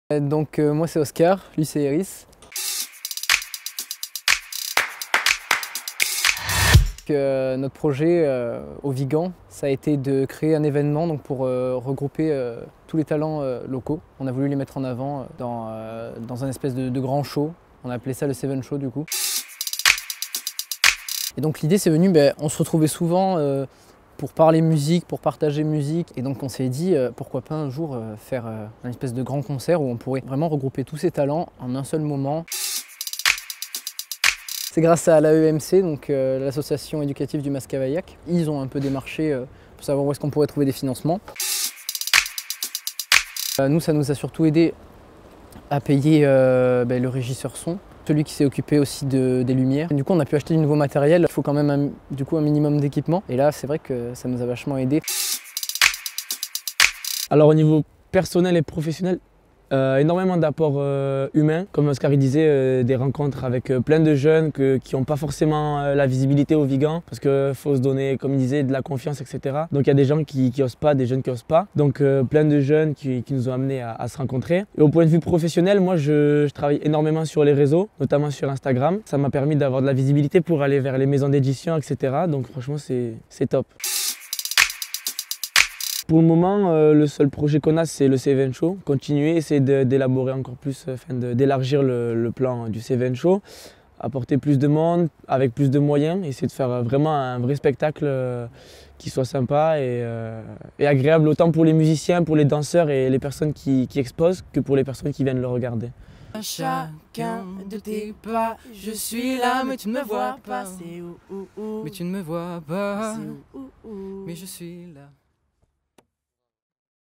Projet Jeunes : Oscar et Airis, lauréats du Projet Jeunes 2019
PROJET JEUNES 🙋‍♂ | Découvrez Oscar et Airis, lauréats du Projet Jeunes 2019. Grâce à l'aide du Département du Gard, ils ont réussi à organiser le «Céven Show» au Vigan, qui pourrait bien devenir un événement culturel majeur au cours des prochaines années, au coeur des Cévennes.
ℹ Le Projet Jeunes est ouvert à tous les Gardois entre 11 et 25 ans.